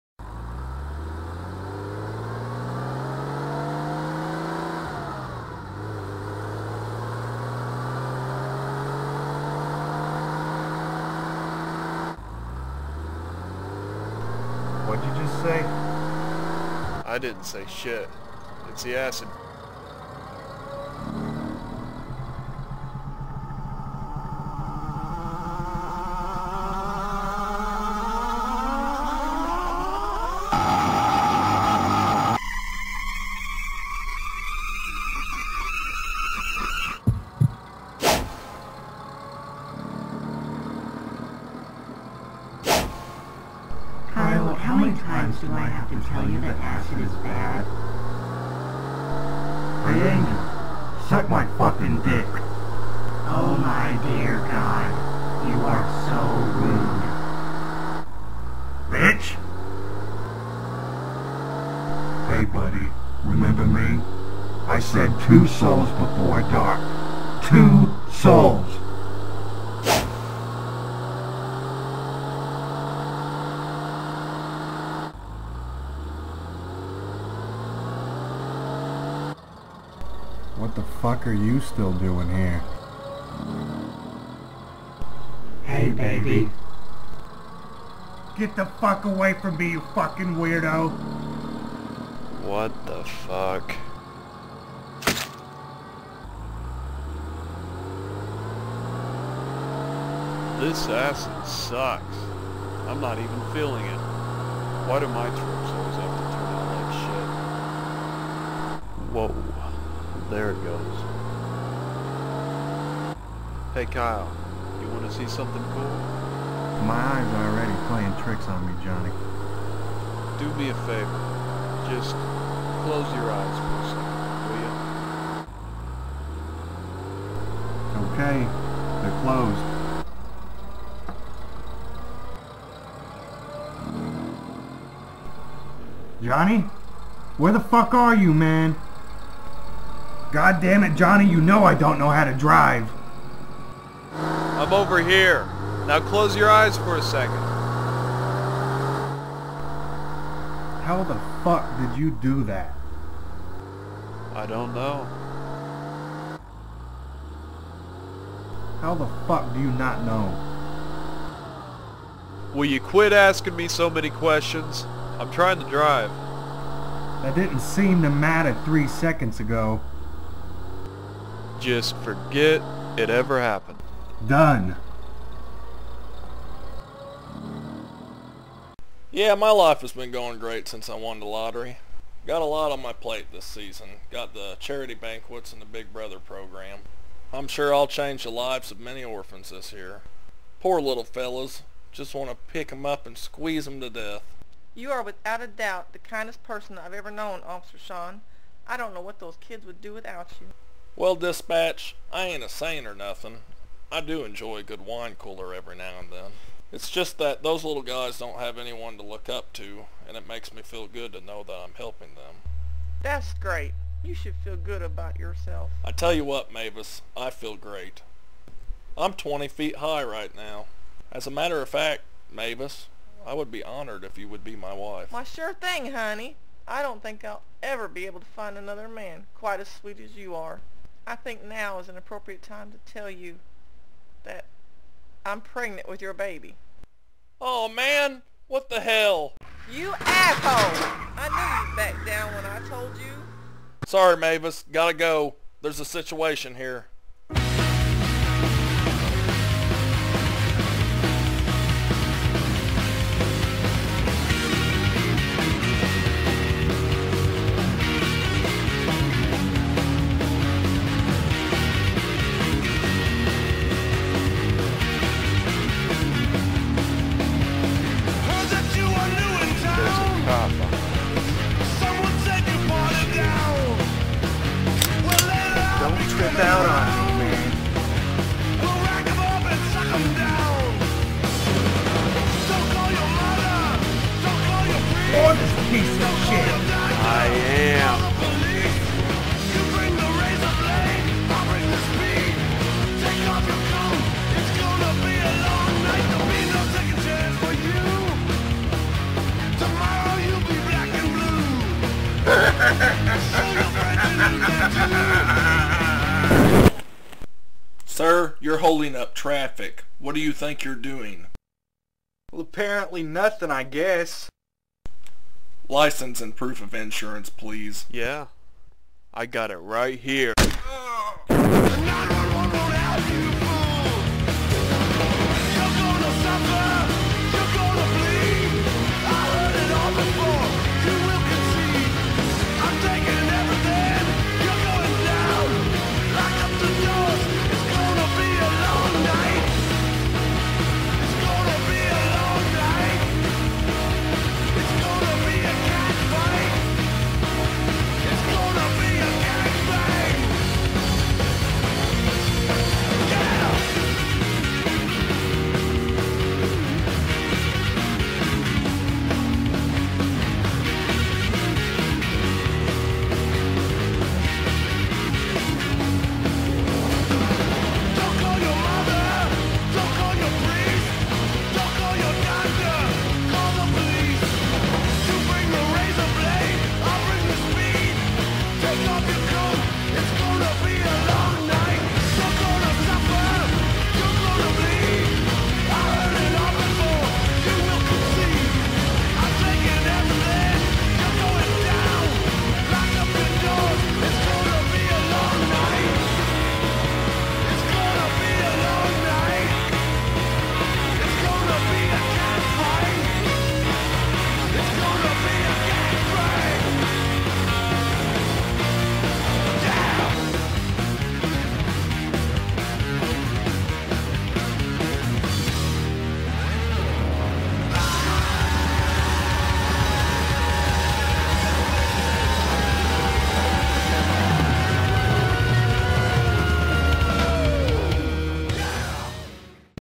Strait to Hell: Ep. 2 - Intro Into the Vaginal Sunset
Johnny and Kyle trip on acid & kill a cop.